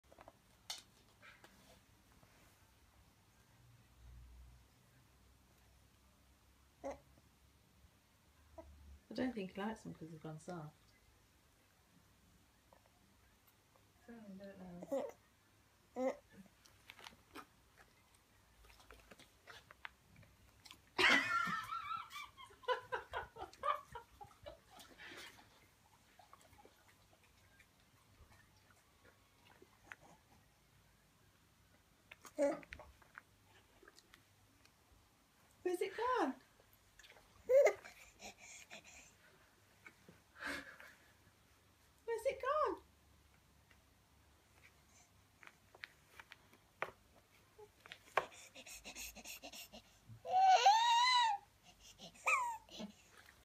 Naughty boxer dog pinches babies biscuit
One of my Boxers Josie taking a biscuit of my unsuspecting grandson Noah, his face is a picture !!